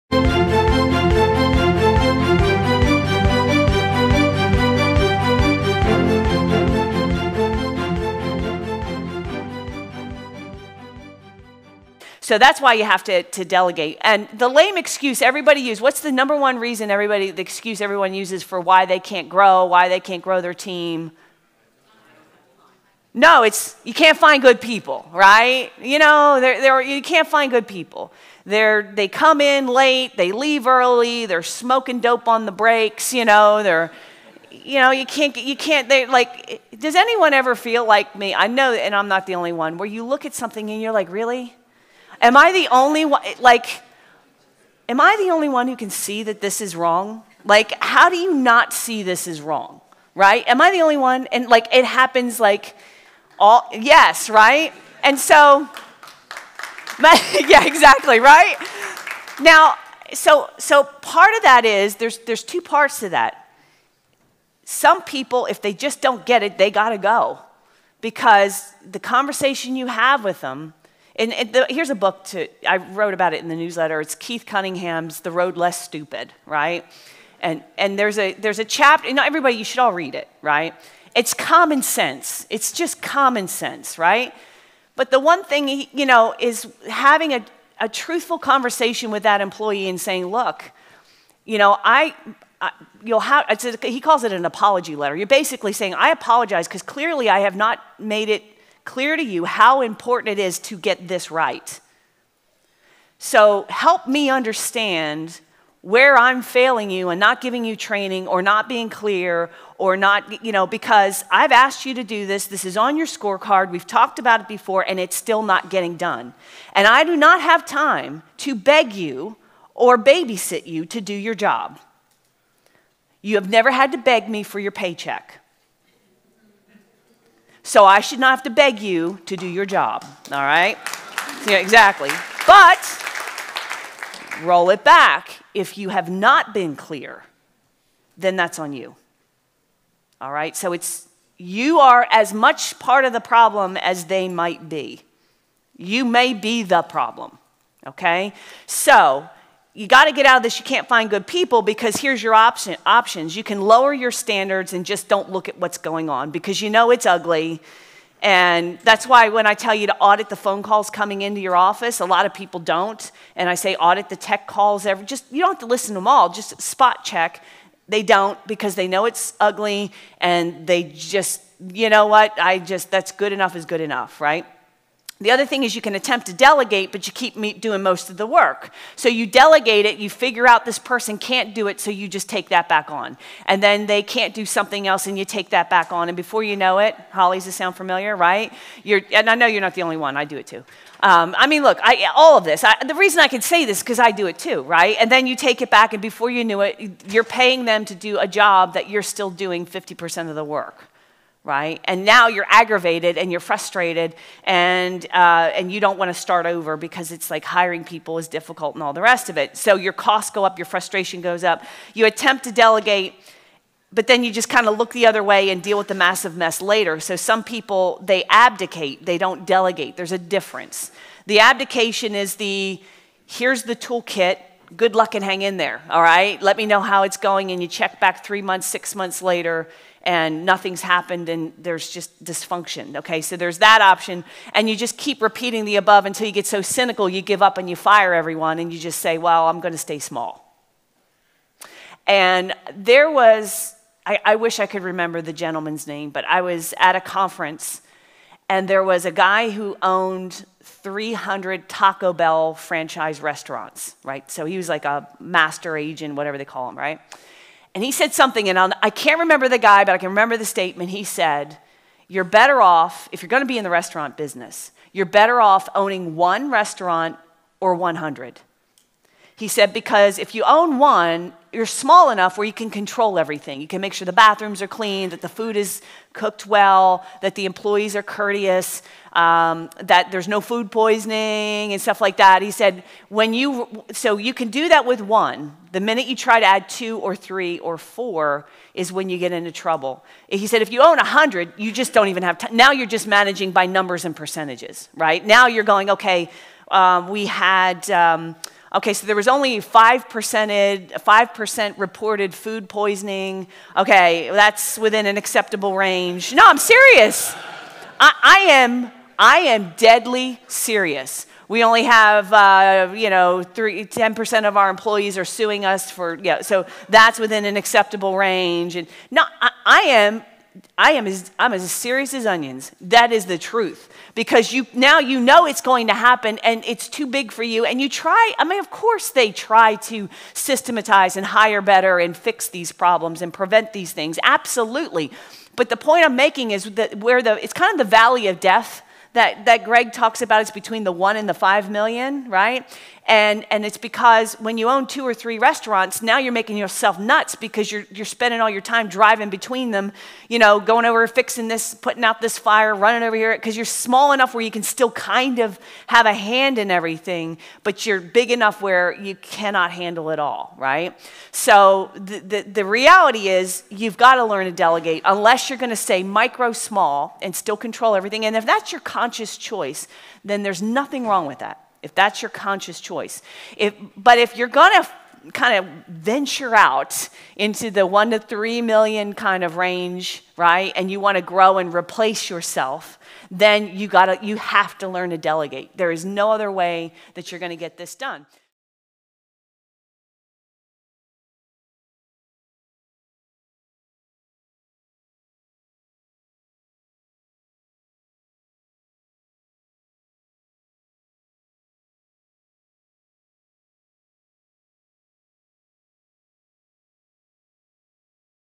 The Lame Excuse MSPs And IT Companies Use For Why They Can't Grow Their Business
What's the #1 reason most business owners give as to why they can't grow their business?

Not enough money? Nope!
Not enough resources? Nah!
Not enough time in the day? Wrong.

In this video, Robin reveals what's holding business owners back from growing their business and how to get around it in 2020.

Want more marketing & business growth strategies?